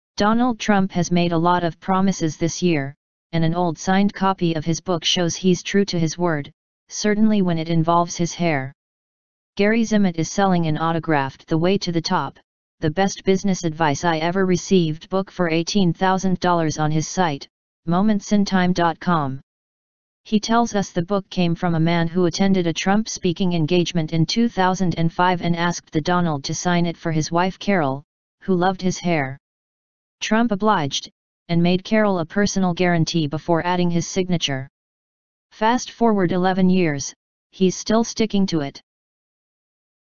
Donald Trump proves he can keep a promise ... at least when it comes to his hair
Donald Trump can keep a promise - $18k Autographed Book Proves It